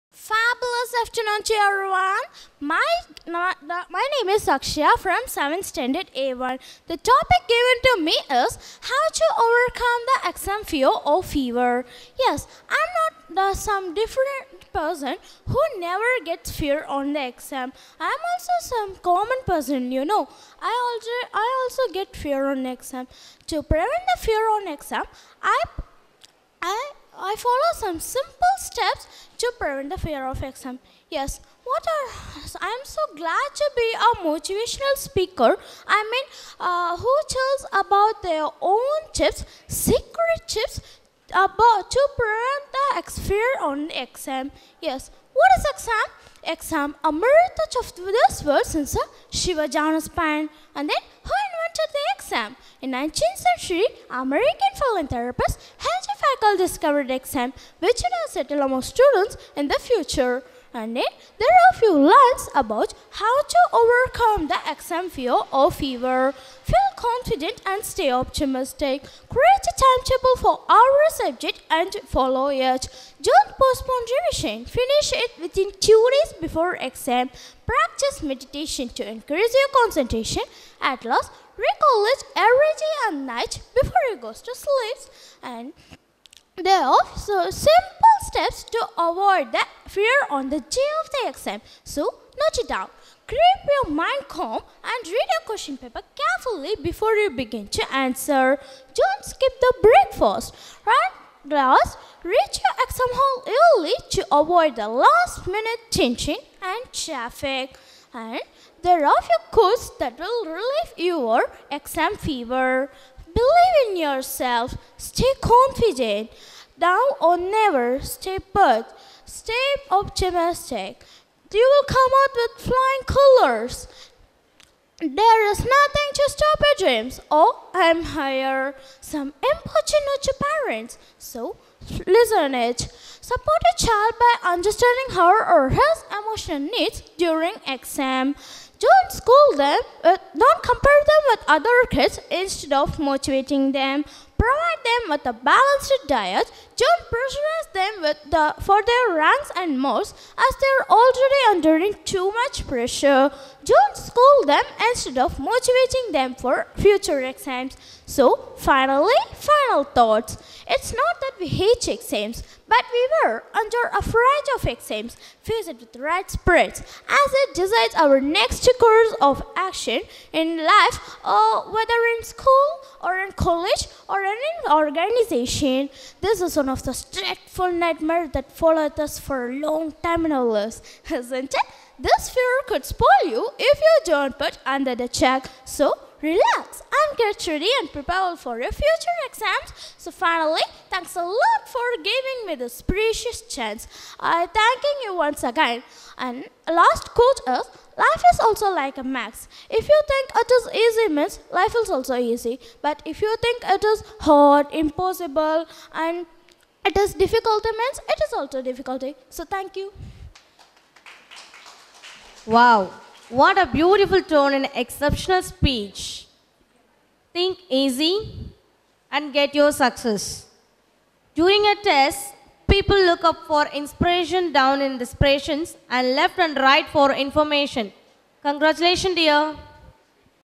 R.Akshaya, Grade-7
"Words have incredible power. They can make people's hearts soar or they can make people's heart sore". Our distinguished competitors (Students) from Grade 7 delivered their speeches in an outstanding way during the oratory competition finale. The students showed great fervor and poise in their presentations. Here we present the glimpse of their speech.